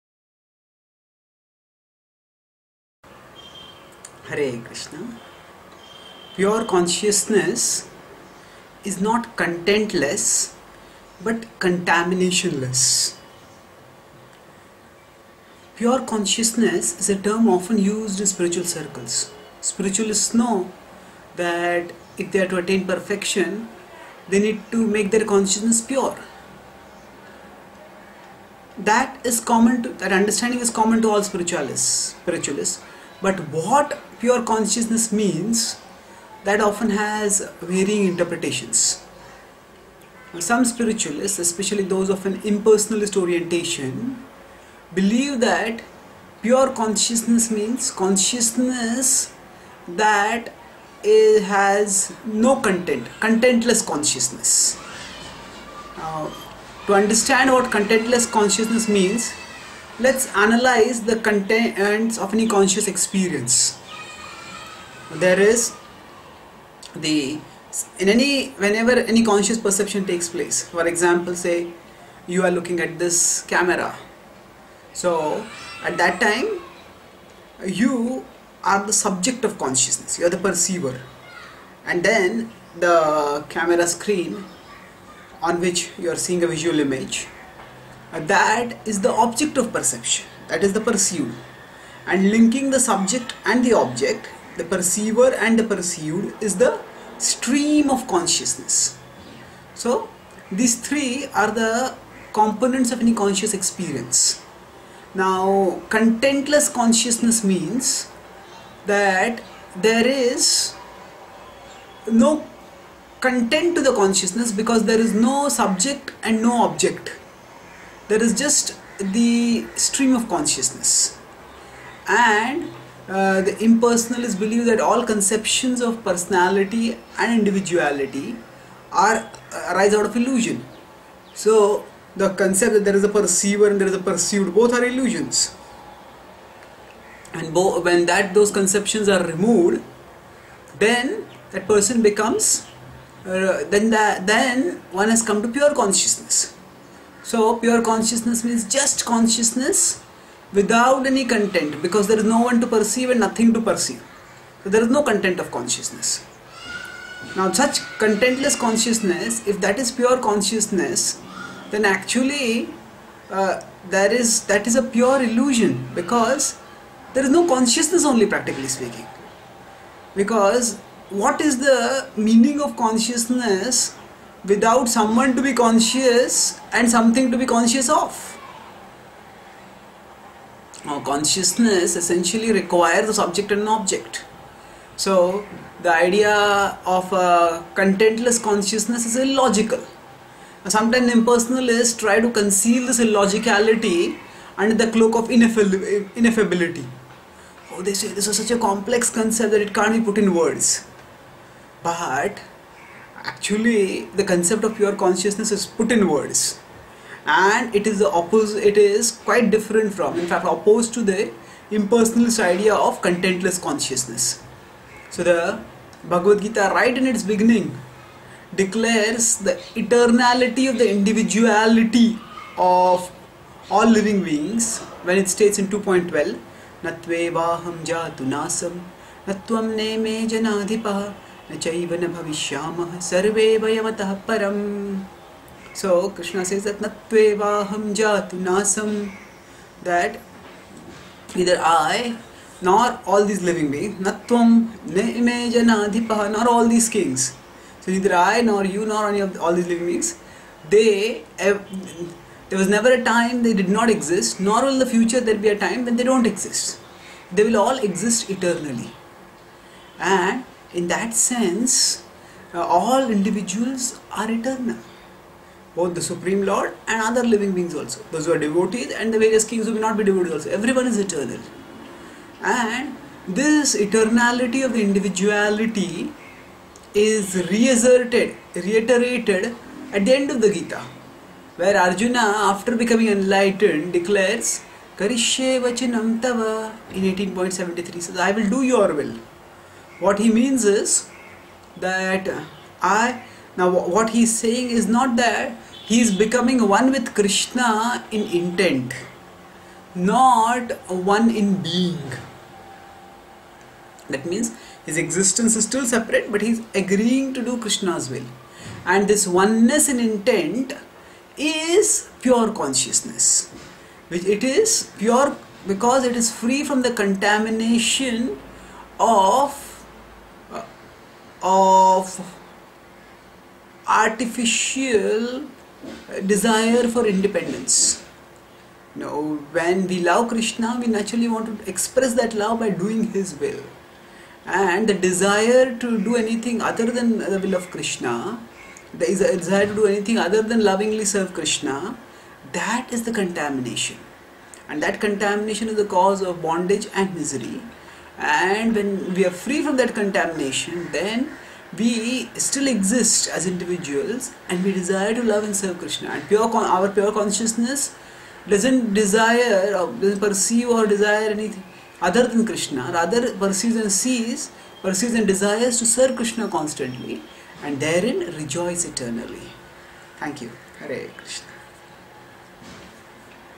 Pure consciousness is not content less, but contamination less
Based on Bhagavad Gita Chapter 02, Text 17, By Chaitanya Charan Das
Article - Pure consciousness is an oft-used term in spiritual circles, but impersonalists interpret it as content-less consciousness.

To understand the problem with such an interpretation, consider the three components of any conscious perception: the subject, the object and the stream of consciousness that links the two. For example, when you read this article, you are the subject, the article is the object and your stream of consciousness links you with the article. Content-less consciousness means that there exists only the stream of consciousness that has no content. There is nothing to perceive and no one to perceive because, according to impersonalists, the notions of subject and object are both illusions.

Such content-less consciousness is, however, consciousness-less, for without a perceiver and experiencer of consciousness, consciousness itself has no meaning or even existence.

Scripture helps us understand the true import of pure-consciousness – it is not content-less, but contamination-less. It is free from any contamination that sullies its nature to perceive and relish pure spiritual reality, reality that manifests at its zenith as Krishna.

In the Bhagavad-gita Krishna (02.11) begins instructing Arjuna by reproaching him for behaving like one in illusion. Then (02.12) he asserts that all living beings – he himself, Arjuna and all the assembled kings – are eternal and eternally individual. Krishna being supremely enlightened is in pure consciousness, yet he perceives others as distinct individuals, who will stay that way eternally. Thus, his consciousness is not or will not become content-less.

And towards the end of the Gita (18.73), when Arjuna’s illusion is dissipated, he resolves to do Krishna’s will. He, the subject, becomes one with Krishna, the object, not in being, but in will. And that oneness is the essence of pure consciousness, which is not content-less, but contamination-less – it is purged of all desires other than the desire to serve Krishna.